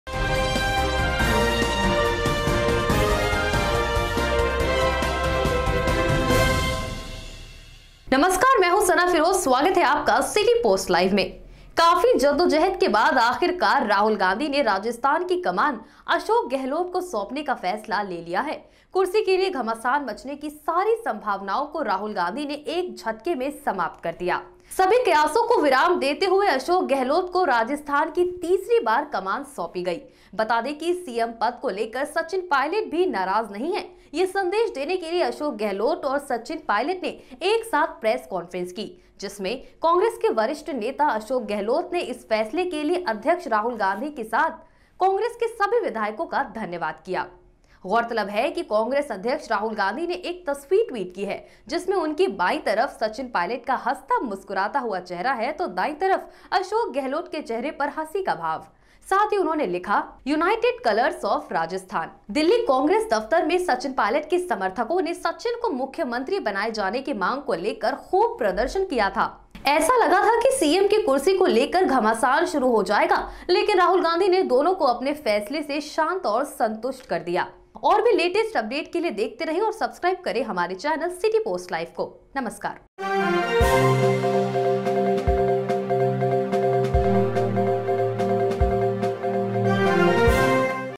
अशोक गेहलोत होंगे रिमोट कंट्रोल मुख्यमंत्री !
काफी जदोजहद के बाद आखिरकार राहुल गांधी ने राजस्थान की कमान अशोक गहलोत को सौंपने का फैसला ले लिया है. ... .कुर्सी के लिए घमशान मचने की सारी संभावनाओं को राहुल गांधी ने एक झटके में समाप्त कर दिया है. सभी कयासों को विराम देते हुए अशोक गहलोत को राजस्थान की तीसरी बार कमान सौंपी गई है. बता दे की CM पद को लेकर सचिन पायलट भी नाराज नहीं हैं, ये संदेश देने के लिए अशोक गहलोत और सचिन पायलट ने एक साथ प्रेस कॉन्फ्रेंस की जिसमे। कांग्रेस के वरिष्ठ नेता अशोक गहलोत ने इस फैसले के लिए अध्यक्ष राहुल गांधी के साथ -साथ कांग्रेस के सभी विधायकों का धन्यवाद करा
. गौरतलब है कि कांग्रेस अध्यक्ष राहुल गांधी ने एक तस्वीर ट्वीट की है, जिसमें उनकी बायीं तरफ सचिन पायलट का हंसता मुस्कुराता हुआ चेहरा है तो दाईं तरफ अशोक गहलोत के चेहरे पर हंसी का भाव. ...साथ ही उन्होंने लिखा की। ‘यूनाइटेड कलर्स ऑफ राजस्थान’.